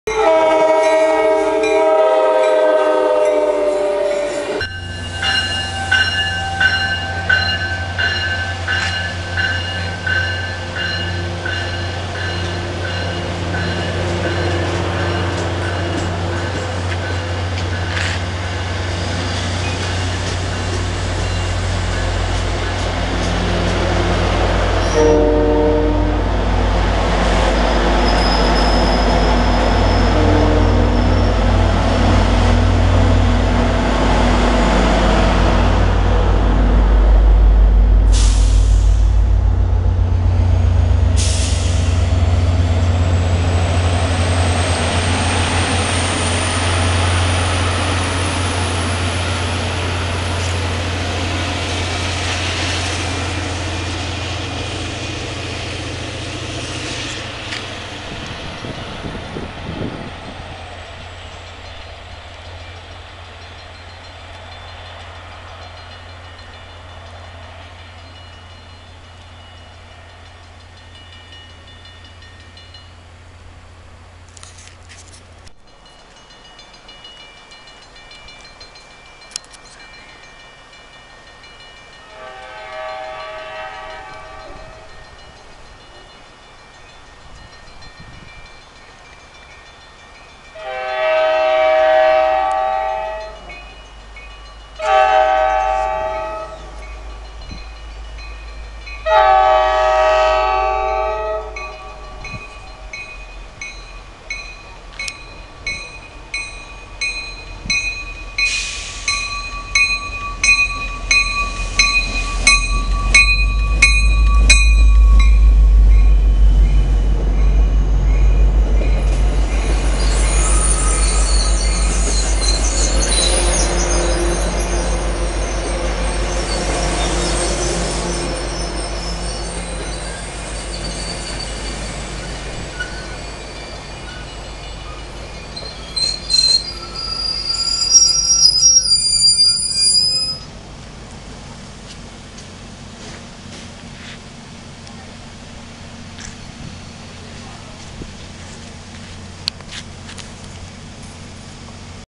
Railfanning at Kingston and Halifax During Evening Rushour 2/3
Both taken on 12/26/13. Both were recorded at Kingston.
1st Train: Train 052 departs at 6:27 PM with 1722, and 1133.
2nd Train: Train 047 arrives at 6:35 PM with 1058 and 1720.
1058 has a great K5LA. Enjoy the video!